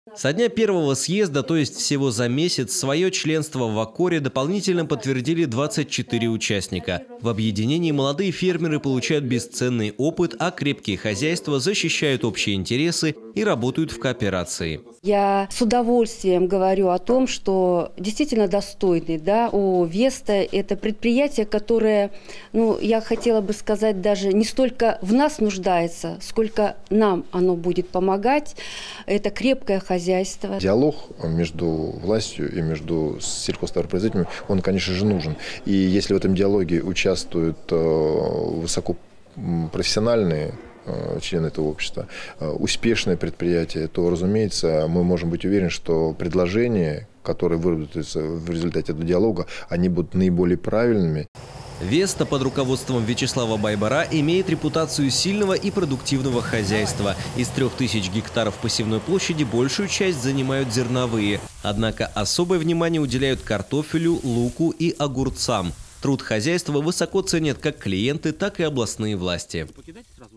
Ассоциация фермерских хозяйств Тамбовской области приняла в свои ряды сотого участника
Со дня первого съезда, то есть всего за месяц, своё членство в АККОРе  дополнительно подтвердили 24 участника. ООО «Веста» стало юбилейным участником АККОР.